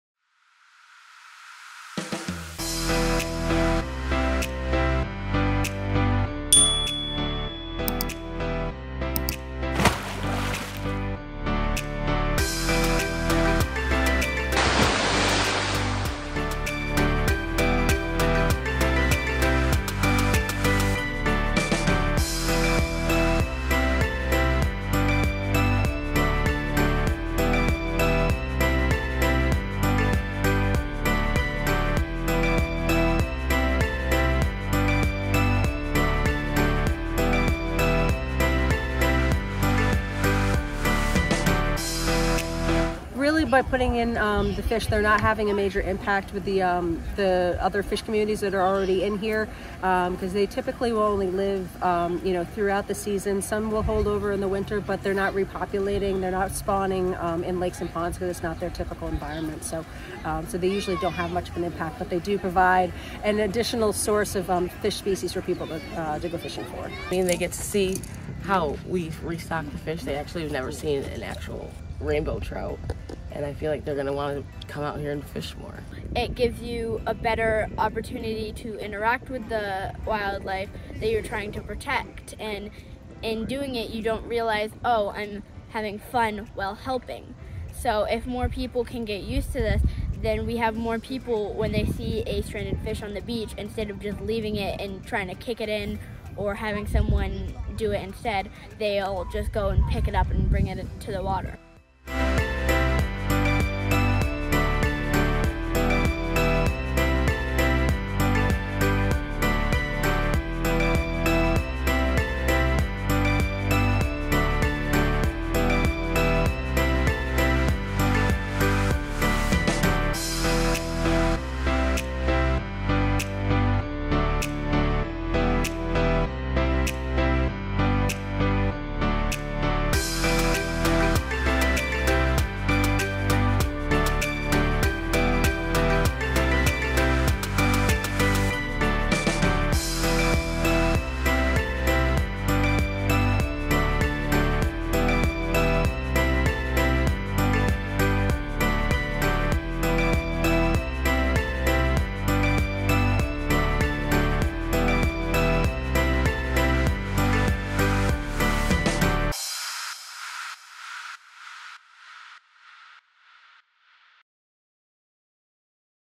Community Members Assist in Trout Stocking Onota Lake
Fish and buckets flew into Onota Lake on Wednesday afternoon, thanks to MassWildlife's annual spring trout stocking event.